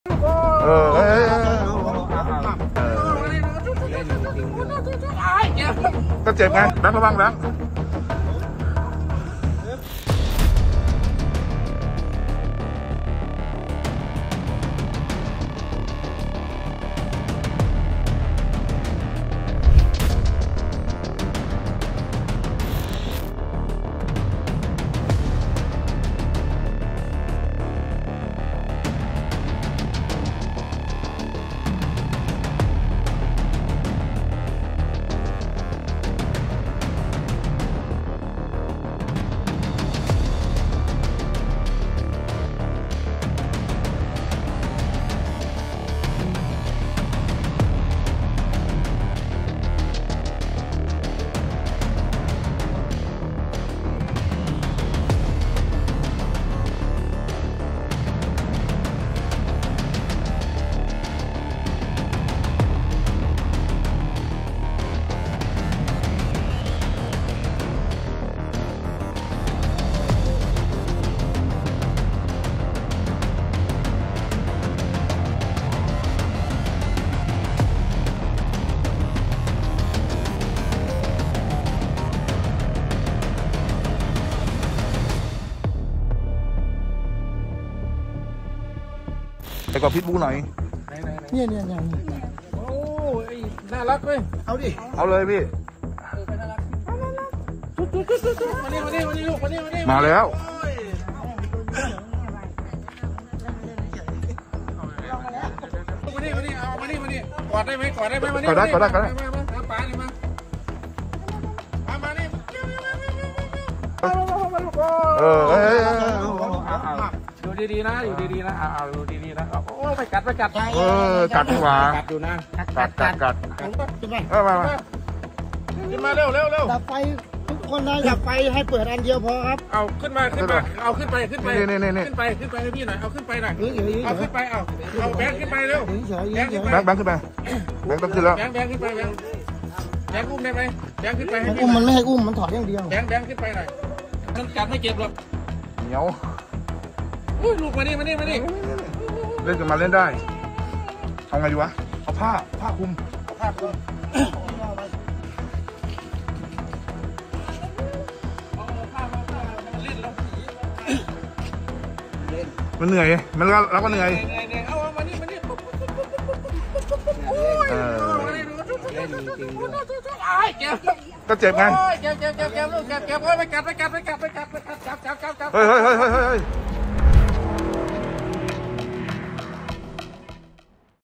ลูกเสือโคร่งโผล่มาจากไหนกลางดึก? ตามจับกันวุ่น! | NewsFeed
ลูกเสือโคร่งโผล่มาจากไหนกลางดึก? ทำชาวบ้านแตกตื่นตามจับกันวุ่น